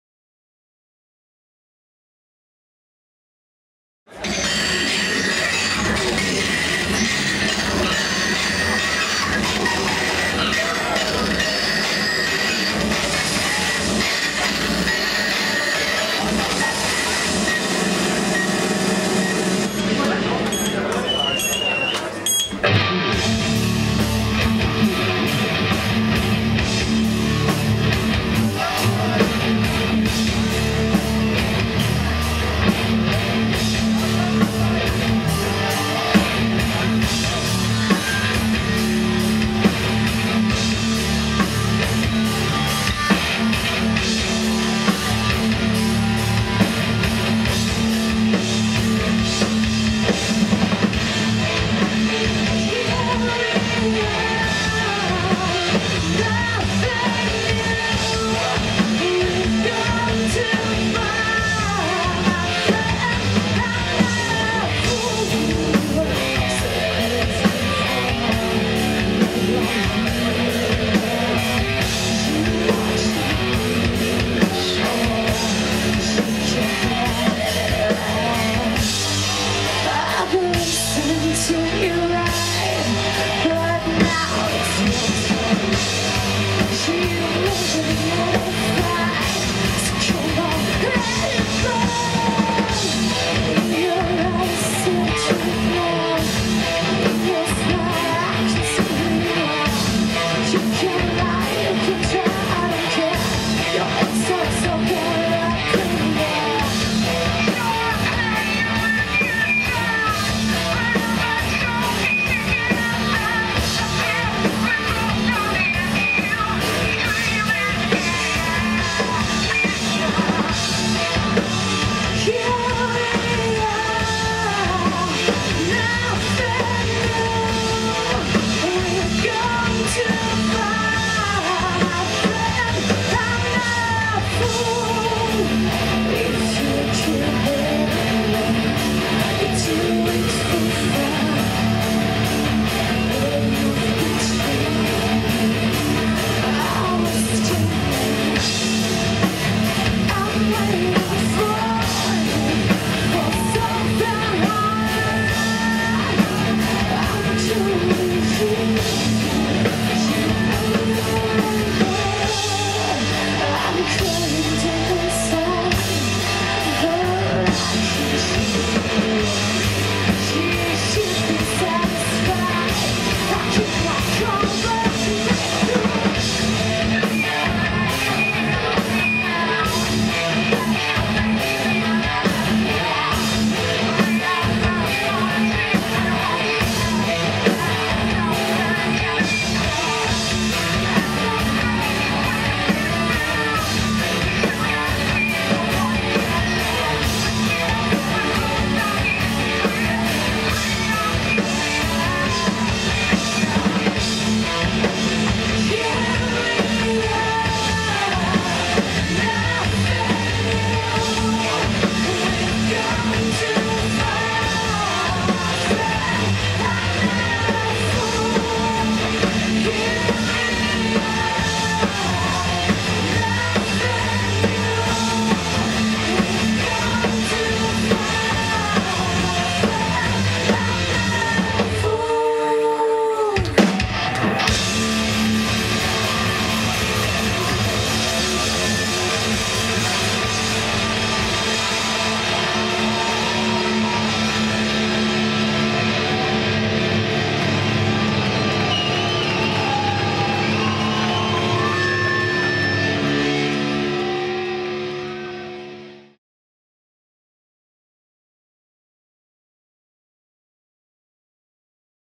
Mingmen live at Rocking Chair
Mingmen, Live at Rocking Chair in Vevey. Performance for the W:O:A Swiss Metal-Battle at the Semi-Final. Filmed by Phil Anderegg and Pascal Gasser for SwissRock.TV